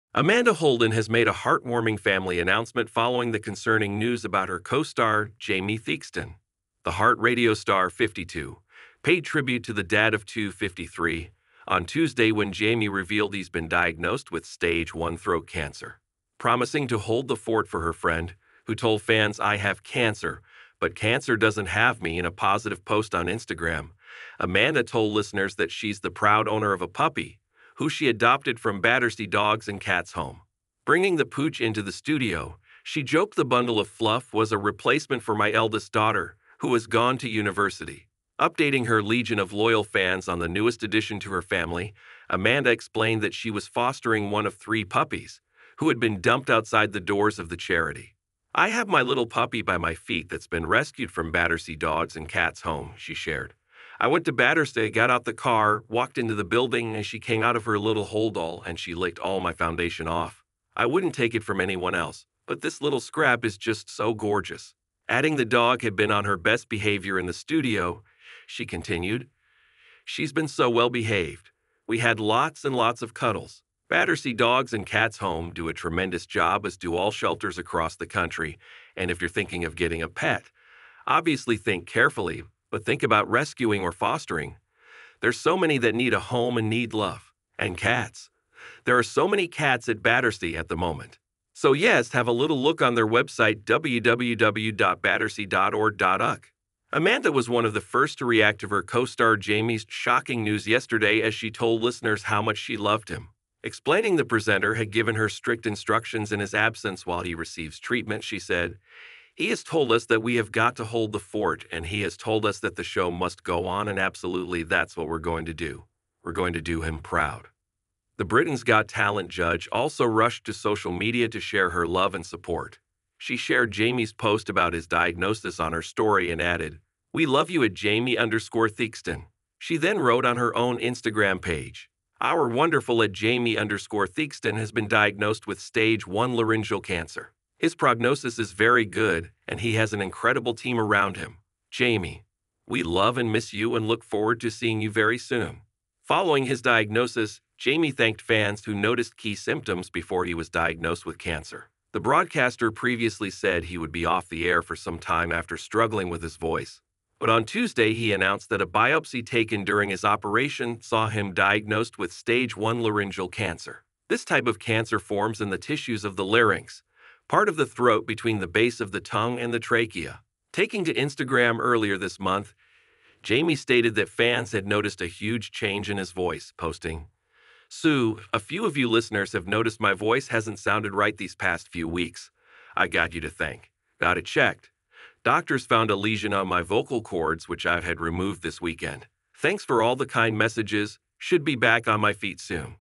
Amanda Holden make sweet family announcement after heartbreaking Jamie Theakston news
Amanda Holden has made a heartwarming family announcement following the concerning news about her co-star, Jamie Theakston.

The Heart Radio star, 52, paid tribute to the dad-of-two, 53, on Tuesday when Jamie revealed he's been diagnosed with Stage 1 throat cancer.

Promising to "hold the fort" for her friend, who told fans "I have cancer, but cancer doesn't have me" in a positive post on Instagram, Amanda told listeners that she's the proud owner of a puppy, who she adopted from Battersea Dogs & Cats Home.

Bringing the pooch into the studio, she joked the bundle of fluff was "a replacement for my eldest daughter ,who has gone to university". Updating her legion of loyal fans on the newest addition to her family, Amanda explained that she was fostering one of three puppies, who had been dumped outside the doors of the charity.